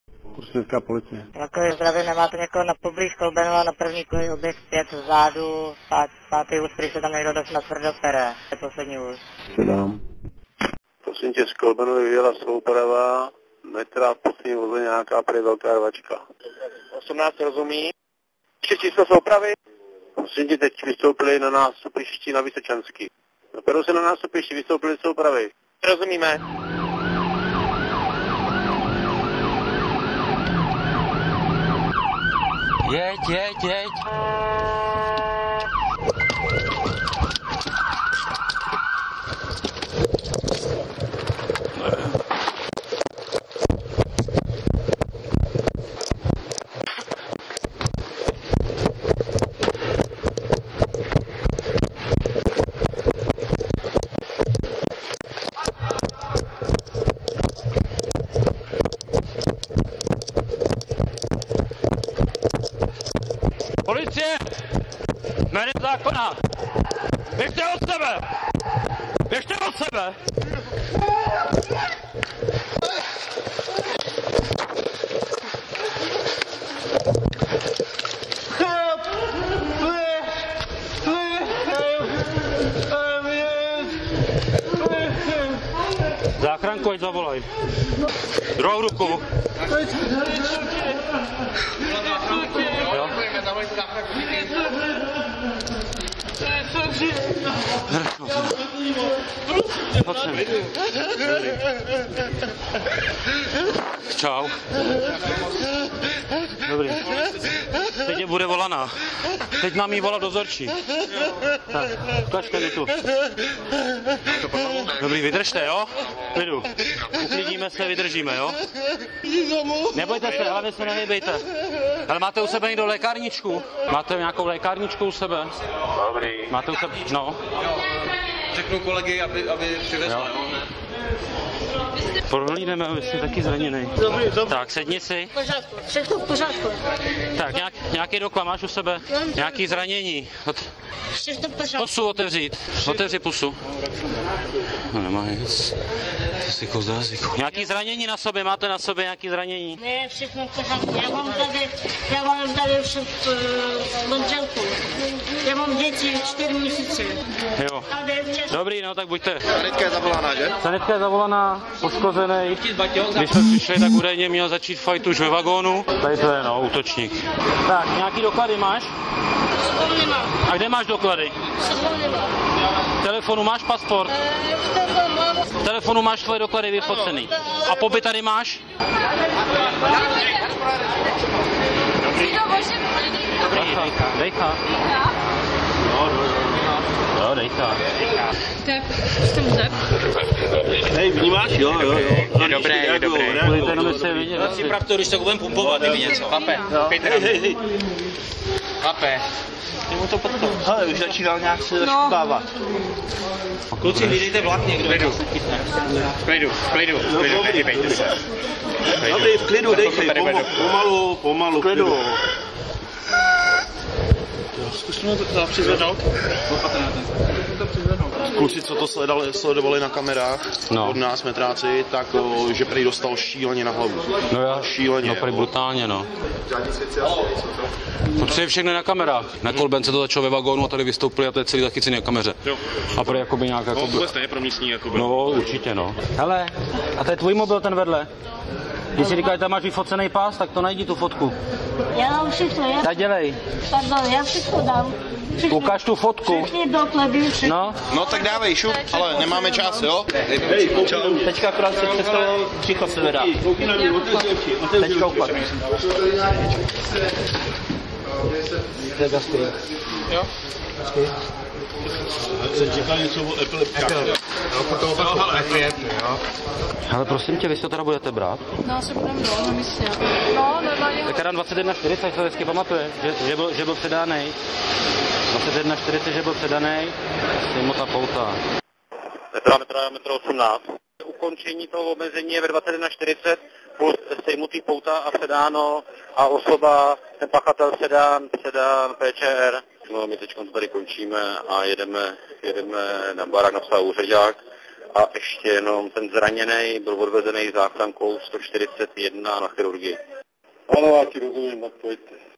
Rvačka a první pomoc v metru
Strážníci zasahovali u rvačky dvou mužů v metru. Zraněný skončil v nemocnici, útočník na služebně PČR.
Oznámení o rvačce dvou osob ve stanici metra Vysočanská převzalo operační středisko 1. března po 21. hodině. Vyslaná hlídka uviděla na nástupišti směrem do centra na zemi dva muže, jeden druhého škrtil. Strážníci útočníka pomocí hmatů a chvatů odtrhli a přiložili mu pouta. Druhému muži, který silně krvácel z obličeje a zadní části hlavy, poskytli první pomoc a přivolali záchrannou službu. Šestadvacetiletého útočníka si převzala státní policie, zraněného 30letého muže odvezli záchranáři po akutním ošetření do nemocnice.